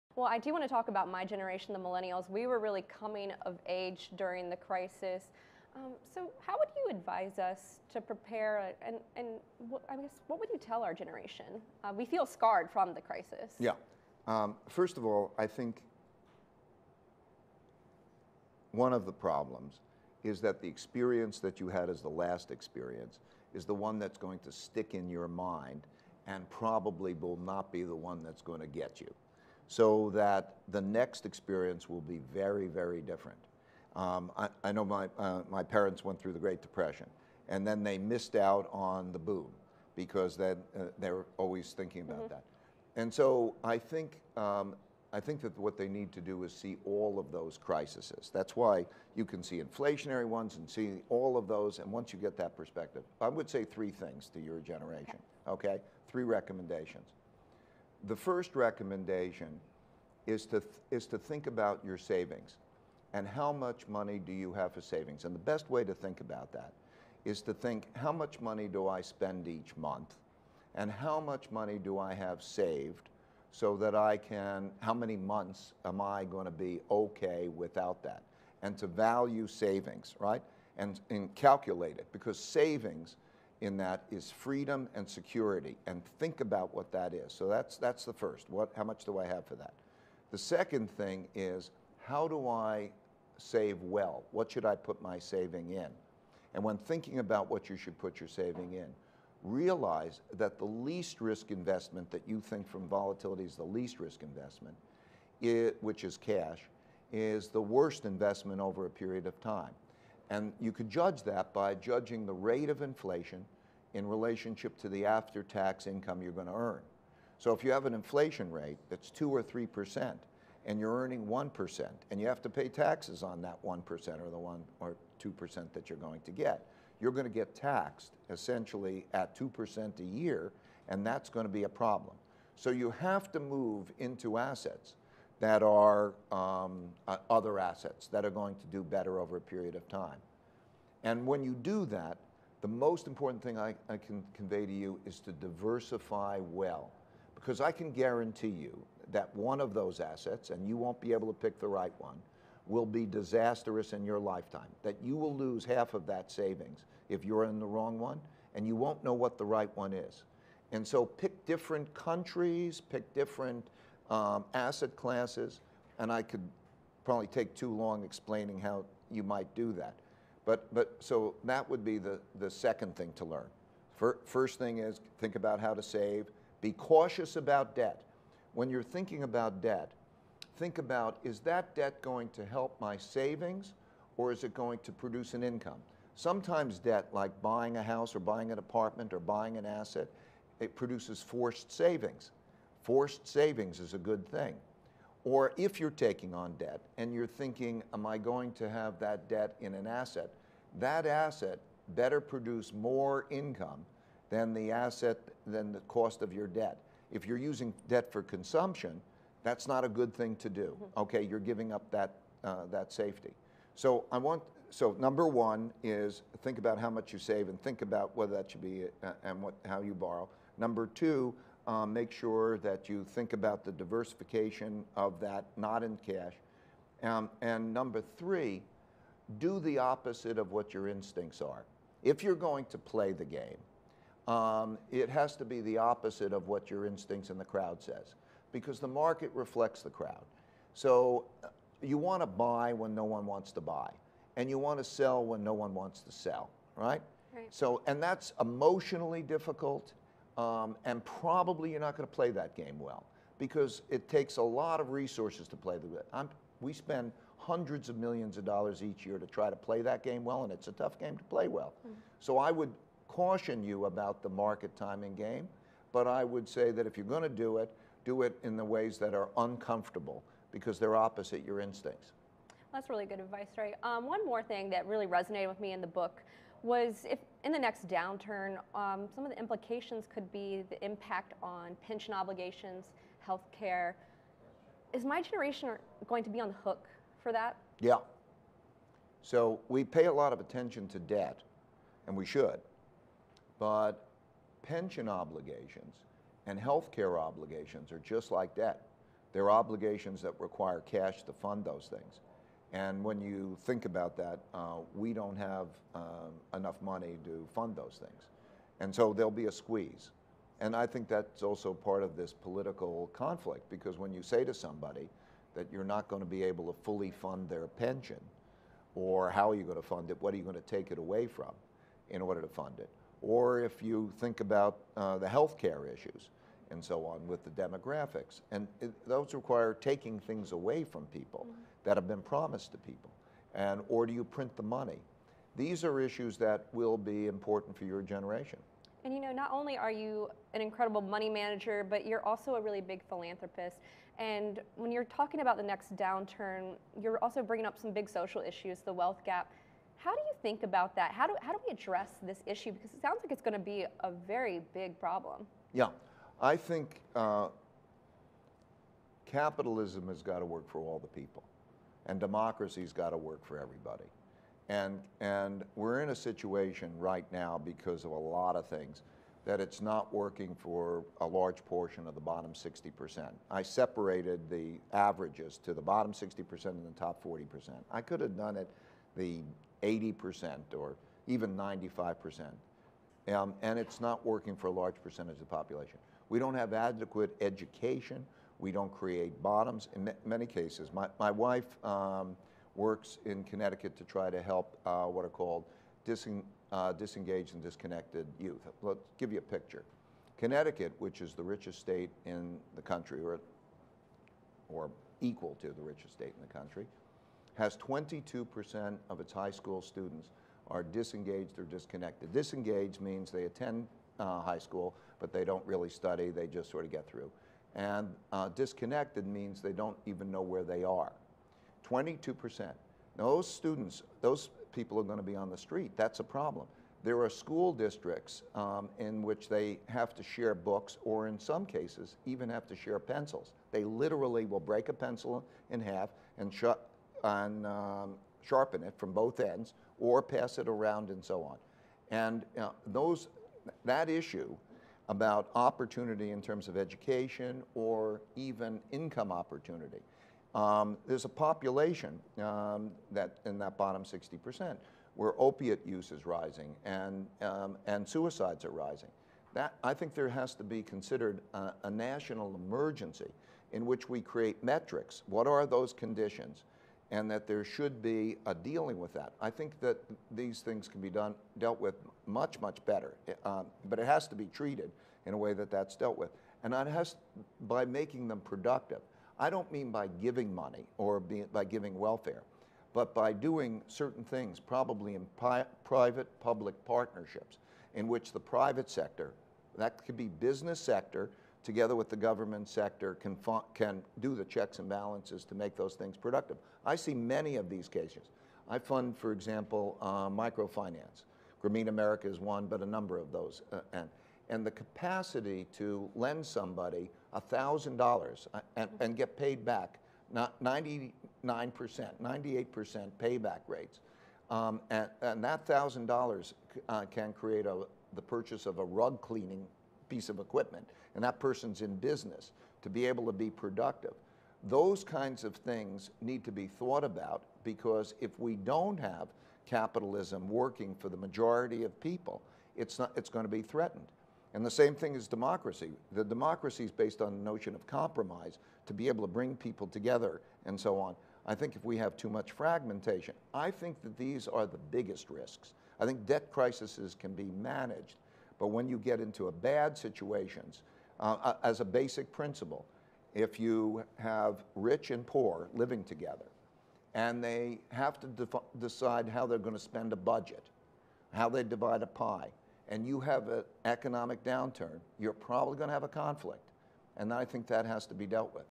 Ray Dalio Gives Recommendations for Millennials
Founder, Chairman and Co-Chief Investment Officer of Bridgewater Associates Ray Dalio talks to Julia La Roche of Yahoo Finance about the value of savings and investing.